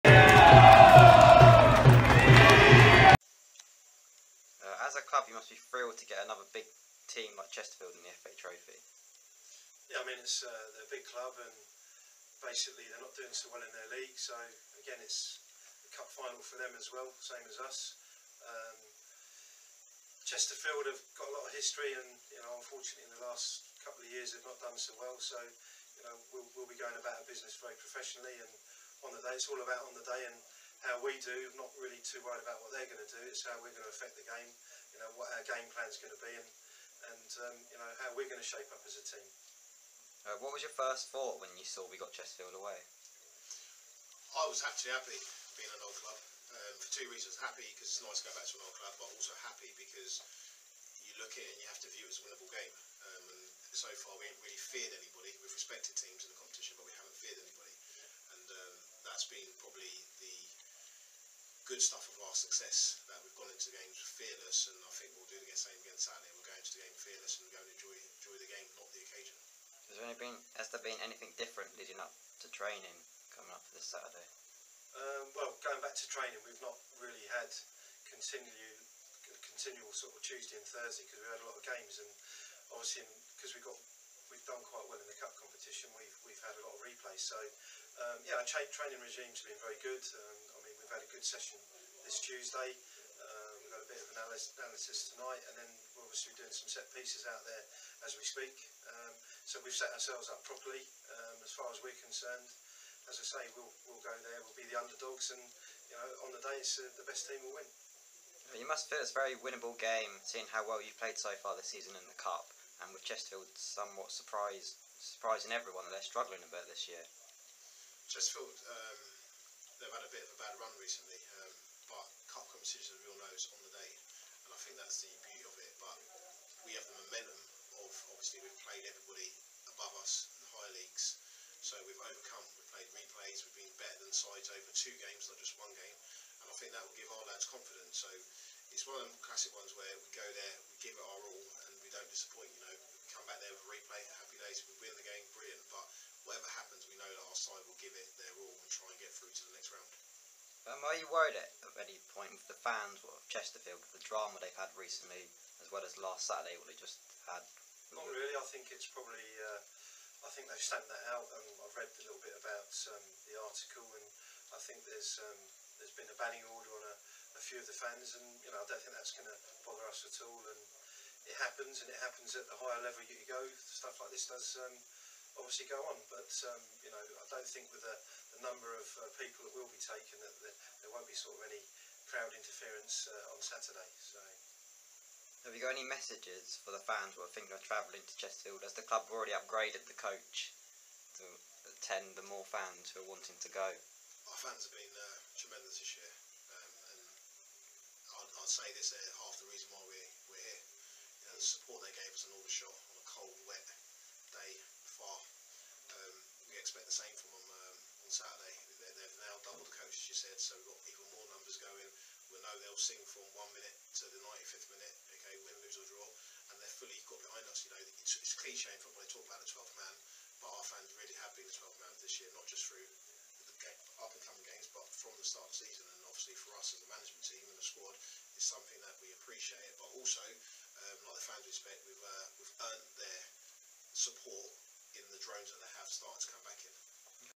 Pre-Match Interview Chesterfield- FA Trophy
Pre-Match Interview with Jon Taylor and Nathan Abby before the biggest game for the club for 19 years in the FA Trophy Second Round 10/01/18